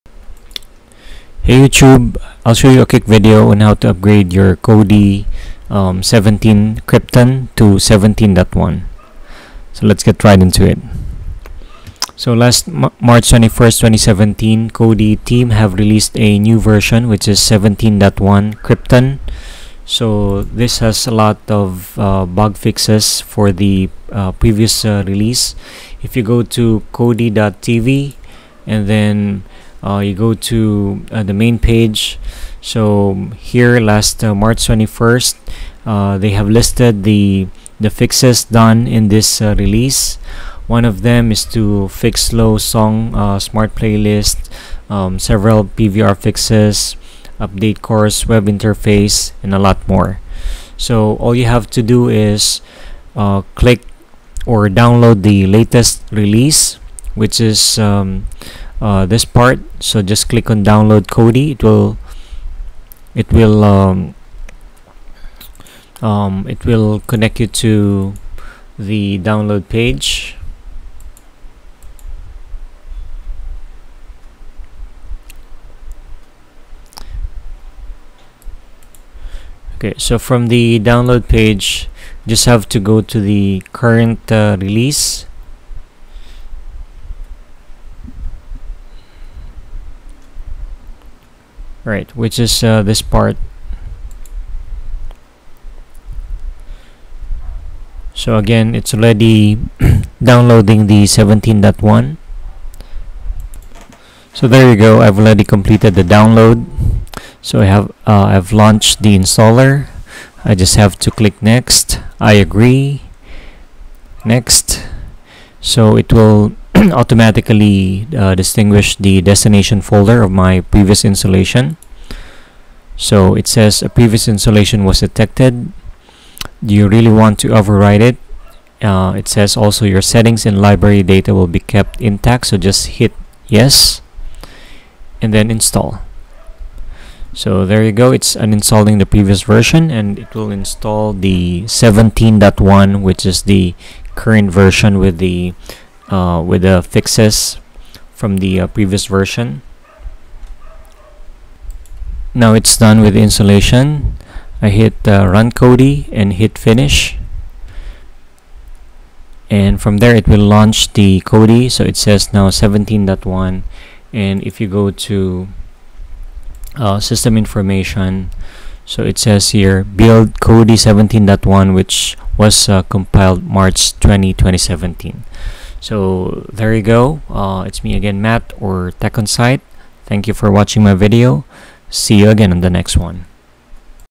How To Update To Kodi 17 1 Current release
This is the bugfix release for v17.1 “Krypton” which contains improvement from the v17 release.